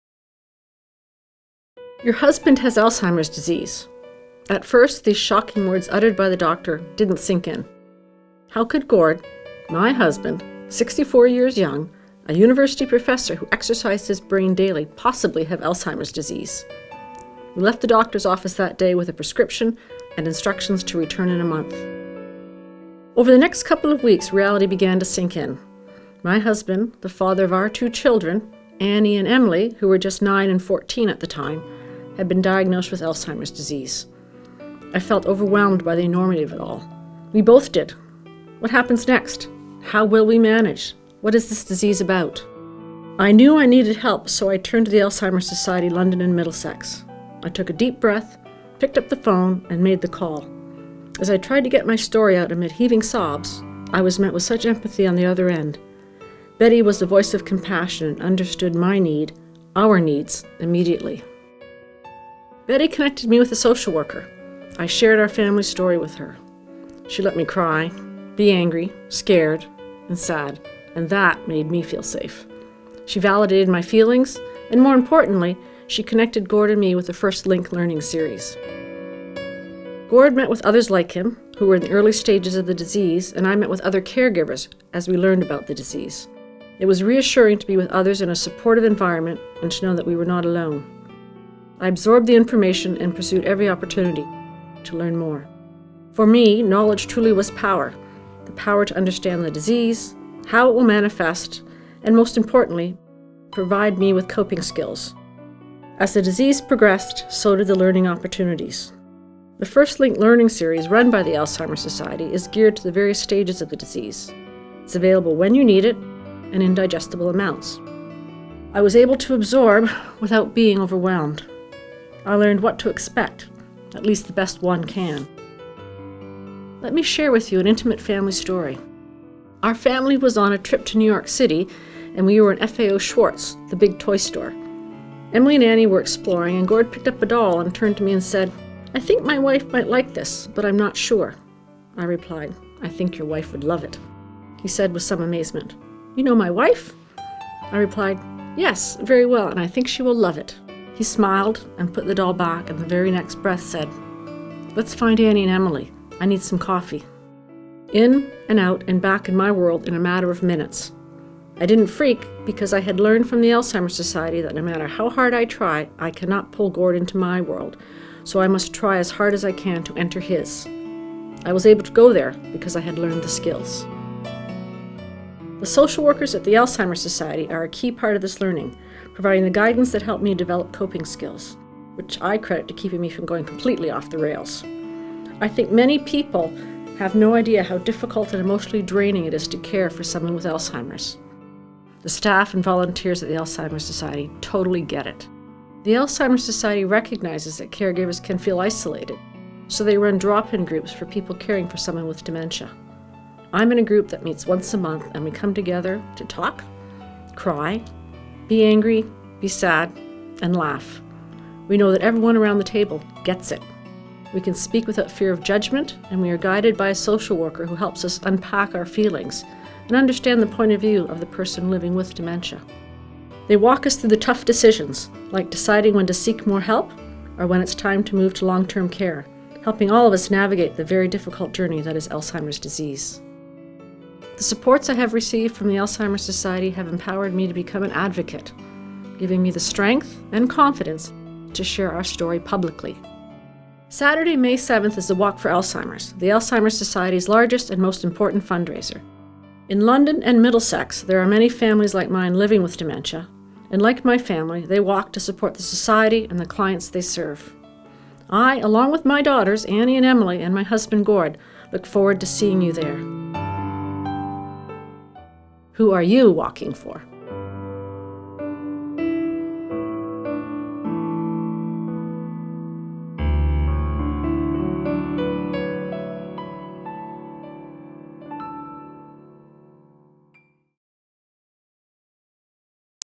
Walk For Alzheimer's 2016 Honouree Family
We are proud to present the McDonald-Milligan family as this year's Walk For Alzheimer's Honouree Family.